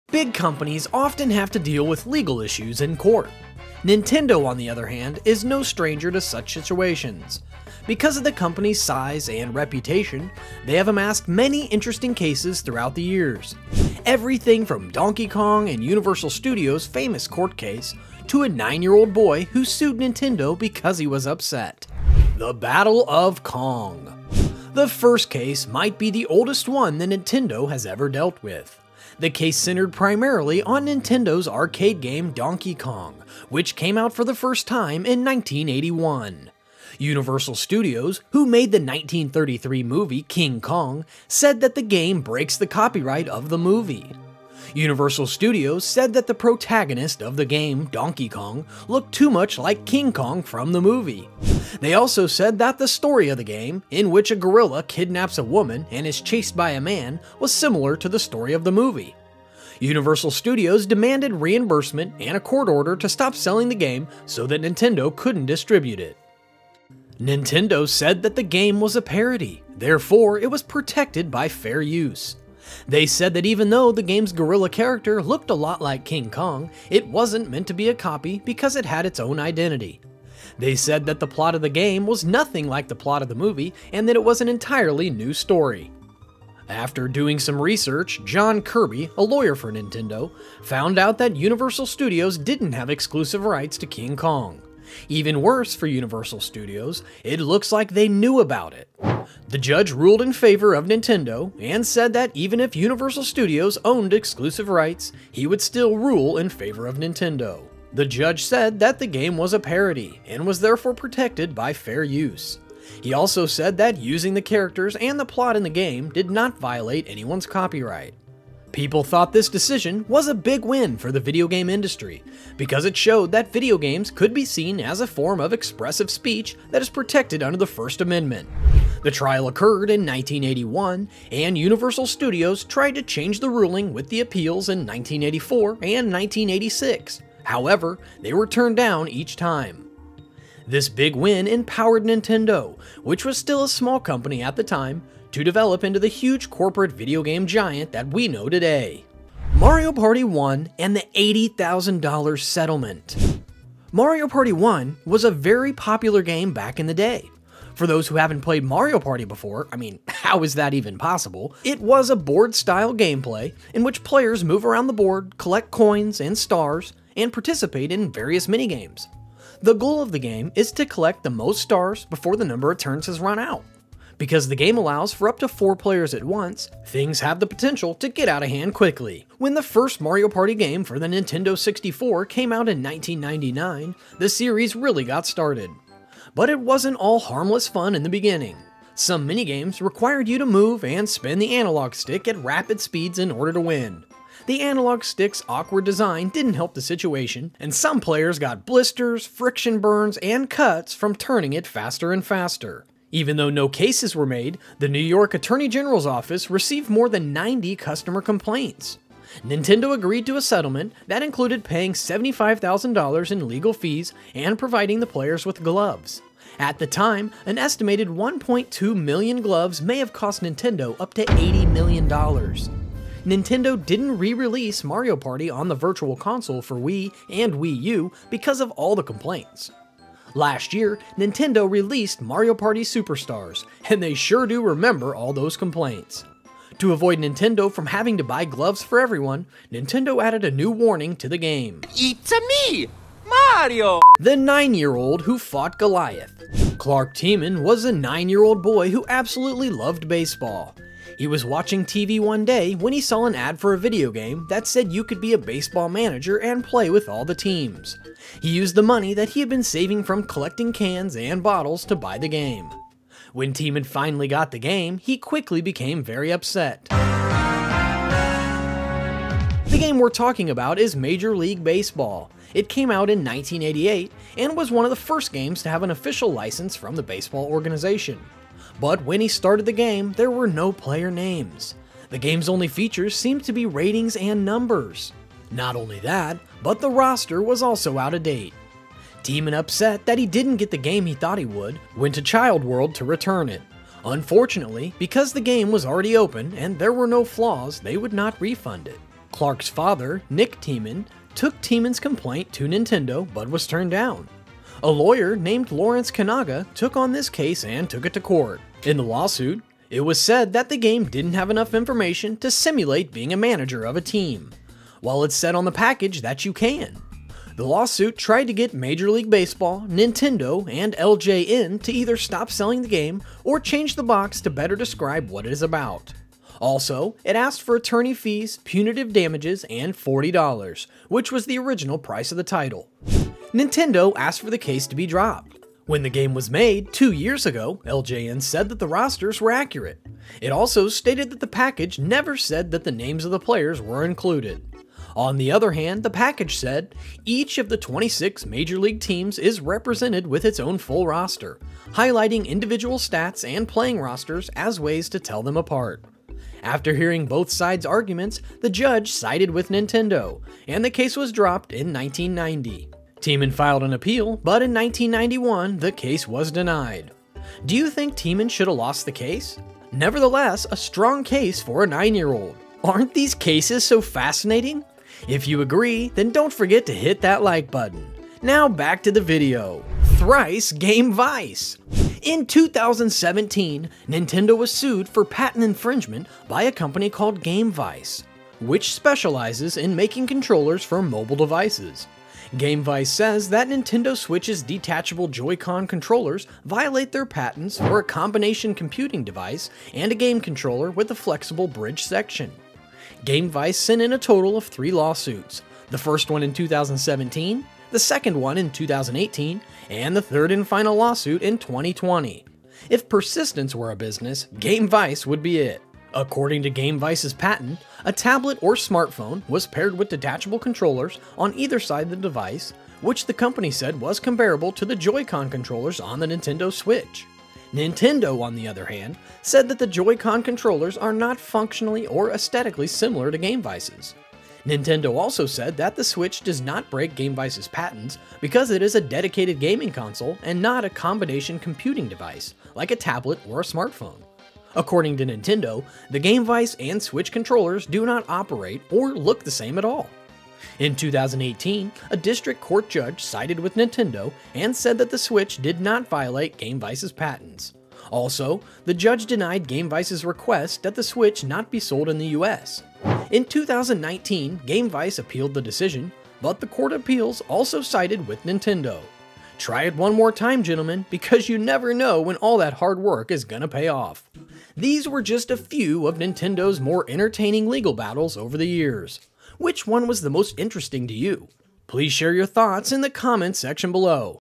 RIDICULOUS Reasons Nintendo Got Sued For
Uncover some of the shocking legal battles of Nintendo! Everything from Universal to a 9-year-old's lawsuit, we reveal the drama behind some of Nintendo's most infamous cases!

▬ Upgrade your Mario Kart skills! ▬
- Mario Kart 8 Deluxe + 12 months of Nintendo Switch
- Nintendo Switch OLED
- Nintendo Switch Pro Controller

▬ Timestamps ▬
00:00 Interlude
00:22 The Battle of Kong
02:29 The $80.000 Settlement
04:04 David & The Goliath
06:19 Thrice Gamevice!